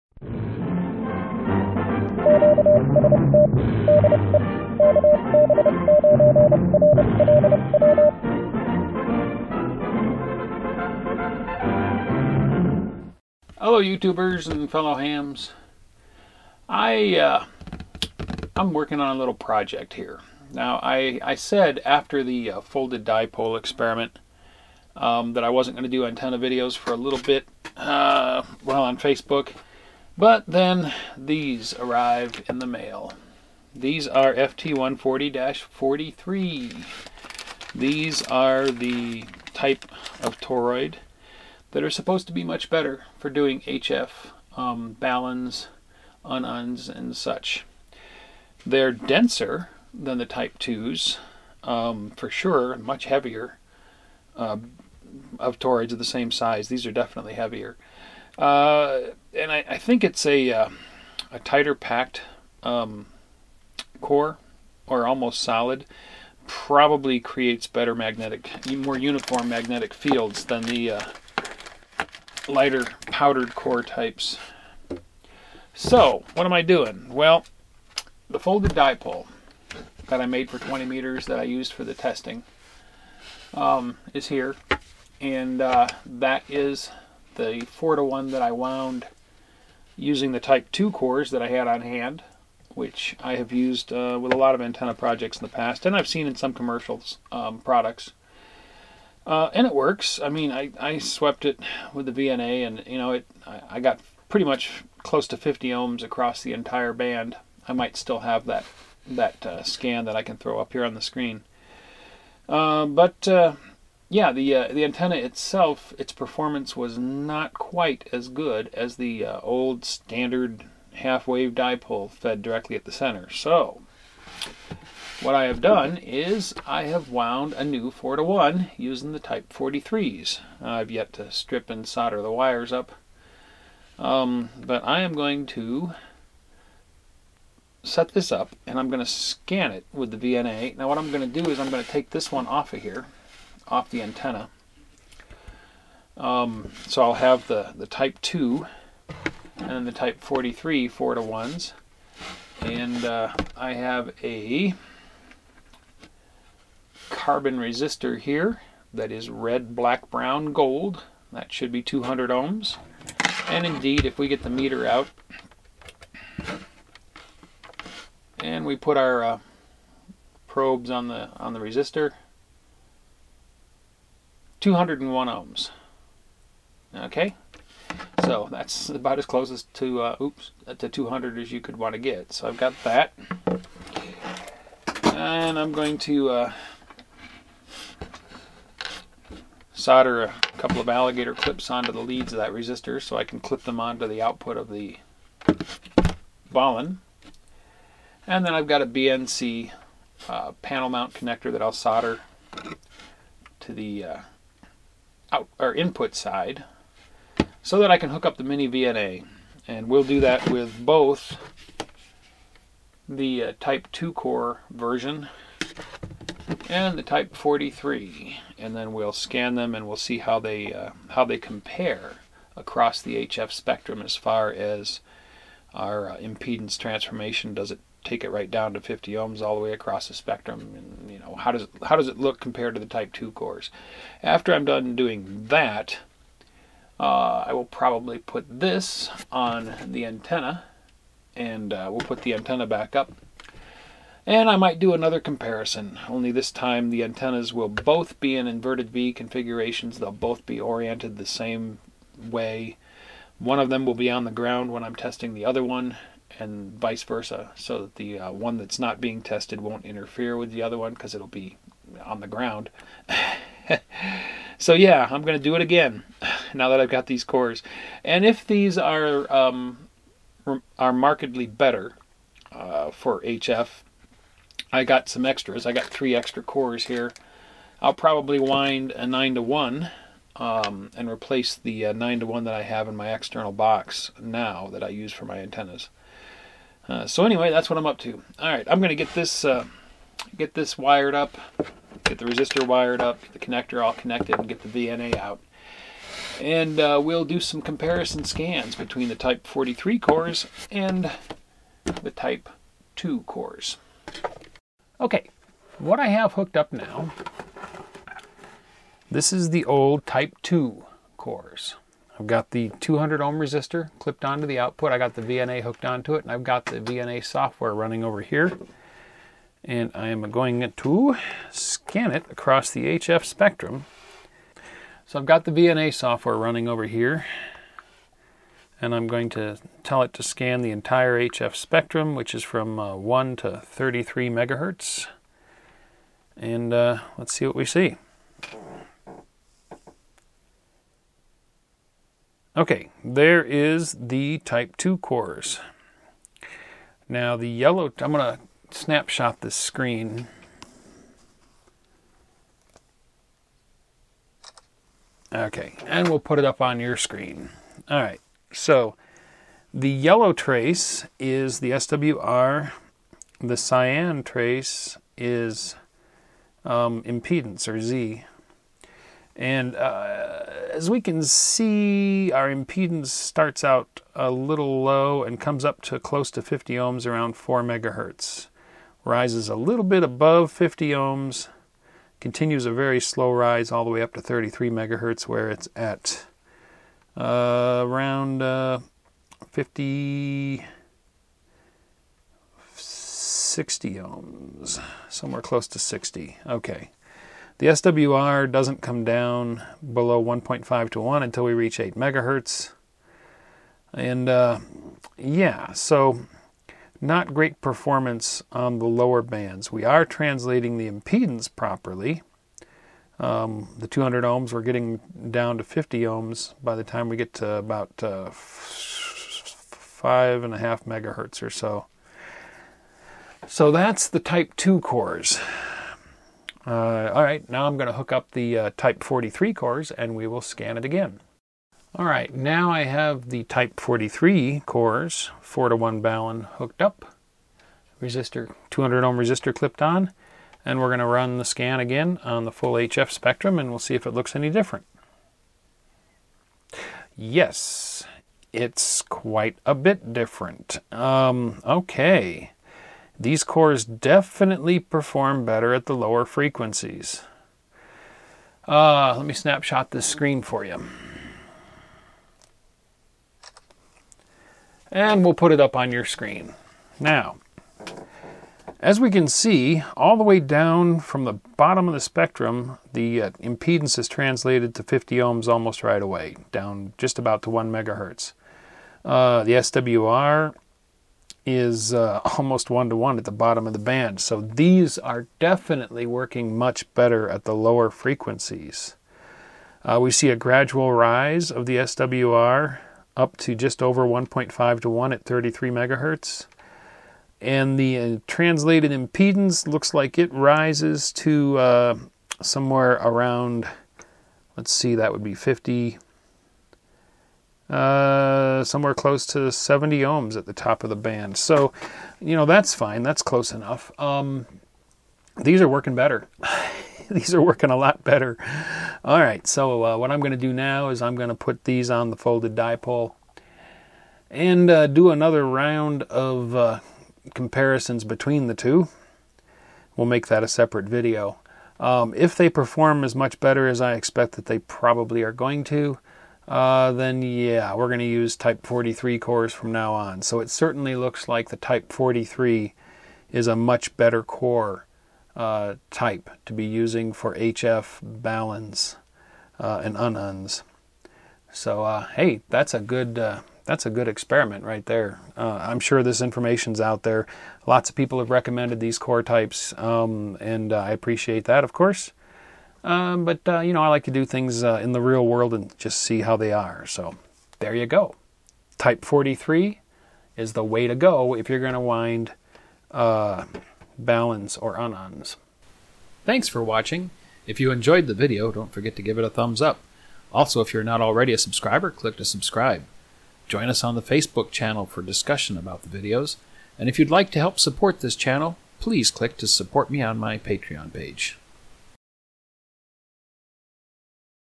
Ham Radio - Which toroid is better for HF, type 2 or type 43? Comparing 4:1 baluns.
I wasn't going to do another antenna video for awhile, then some toroids showed up in the mail.  So, I compare 4:1 baluns wound with type 2 and with type 43 cores.